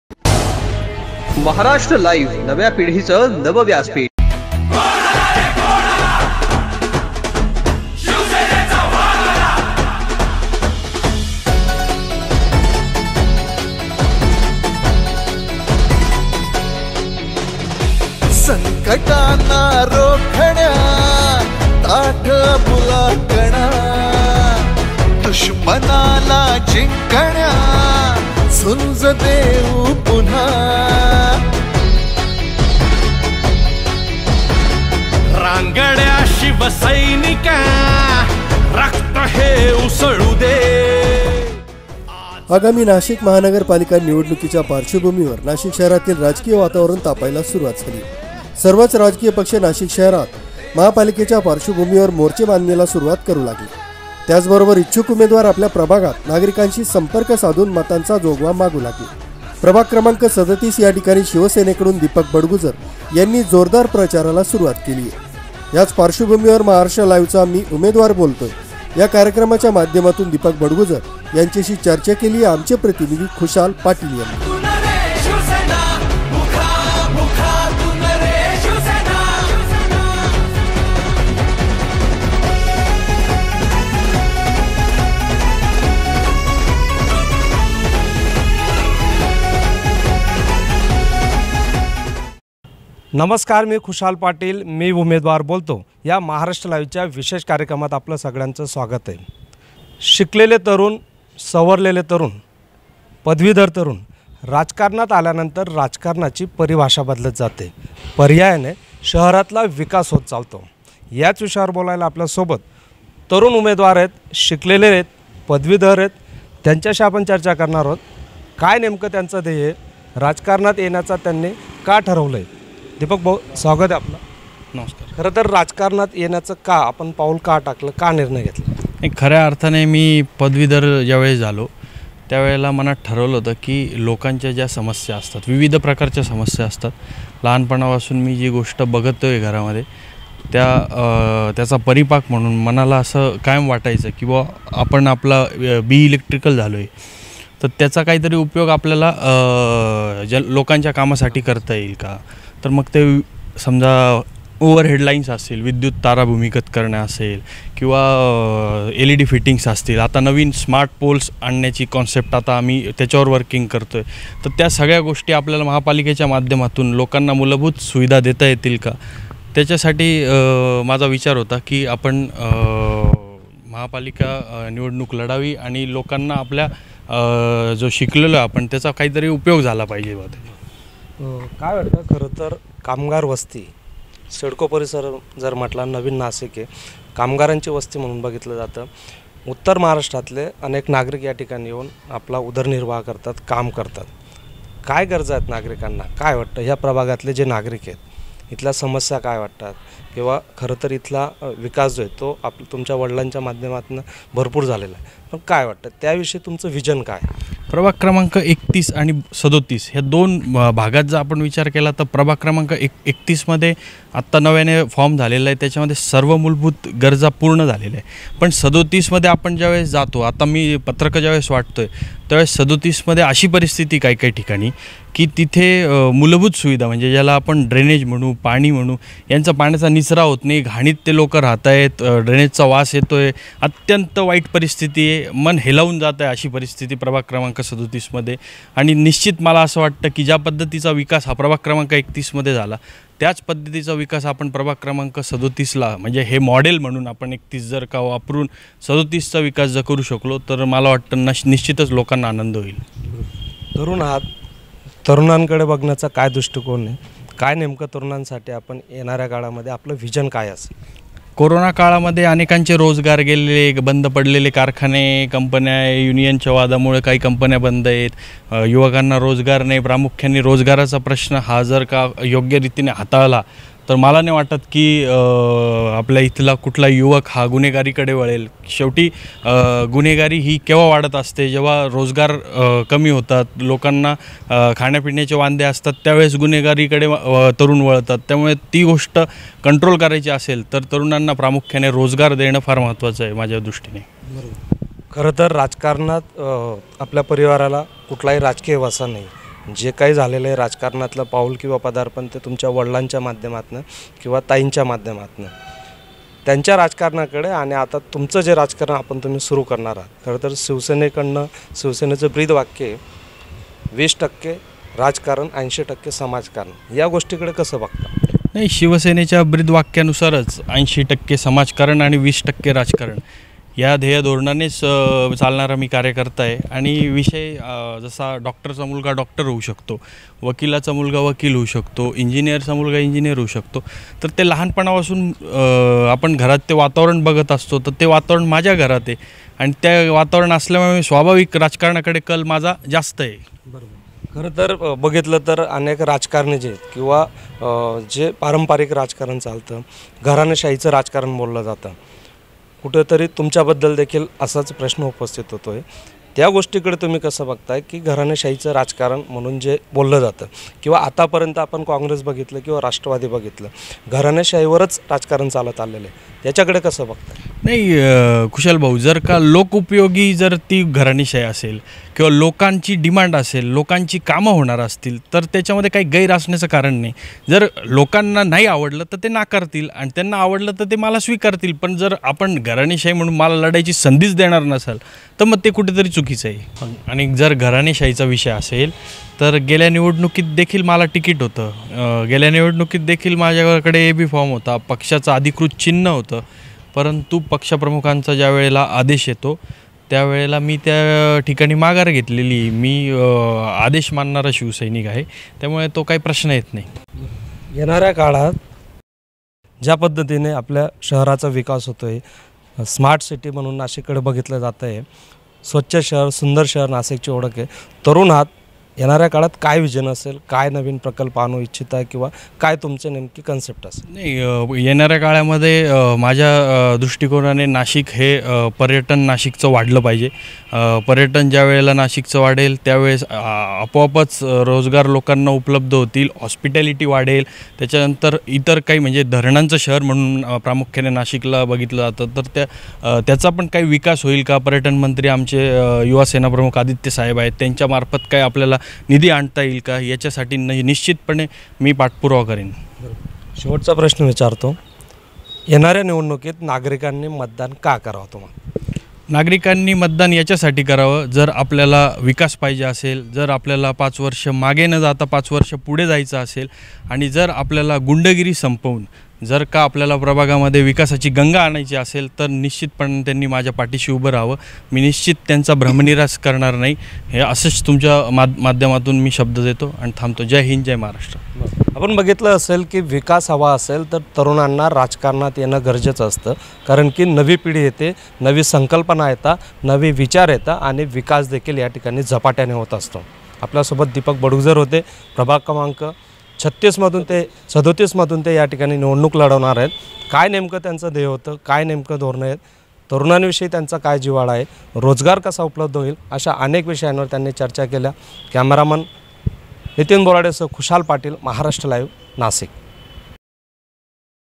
'मी उमेदवार बोलतोय' : प्रभाग ३७ मधून दीपक बडगुजर यांच्याशी चर्चा...
प्रतिनिधी खुशाल पाटील नाशिक

Maharashtra Live , India's first 24-hours Marathi news portel channel, which offers objective news coverage.